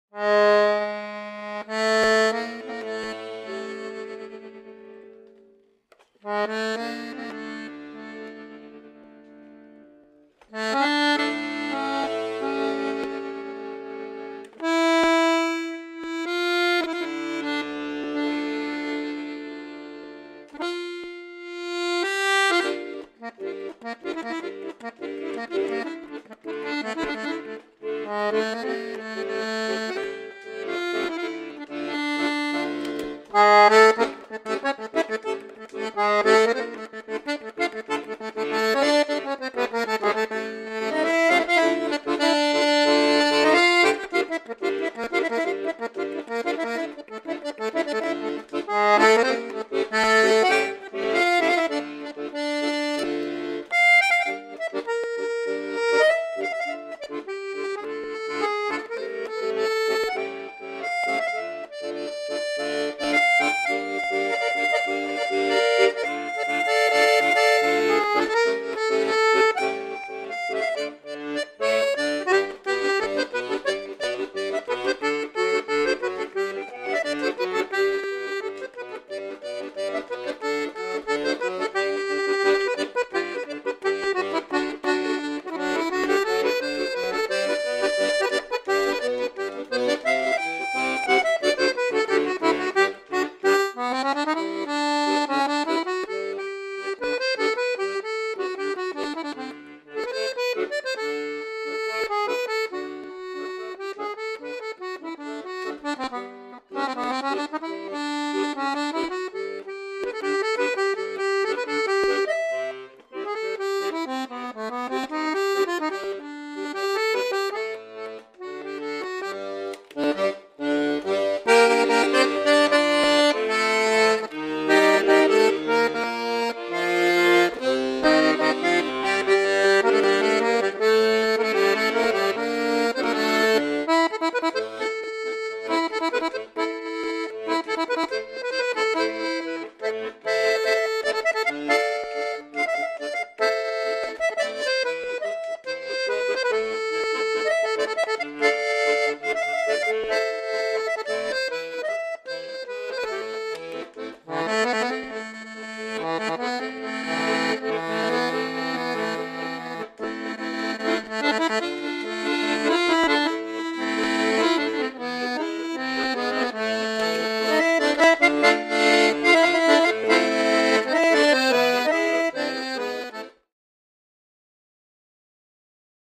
6267 - Candy Stripe Salanti Piano Accordion LM 41 120 $799
Brand:  Salanti
Model: Salanti
Type: Piano Accordion
Reeds: 2/4 LM
Treble: 41 Keys, 14.75"(37.5cm) Key to Key, 3 Registers
Bass: 120 Bass Buttons, 2 Registers
Weight: 15.5lbs / 7kg
Features: Ultra compact and lightweight in cream puff condition
Includes: Used Straps and Case, 6 Month Store Warranty
Suggested Upgrades: New Straps, New Case, Professional Microphone Installation, Accordion Stand, Accordion Spray Polish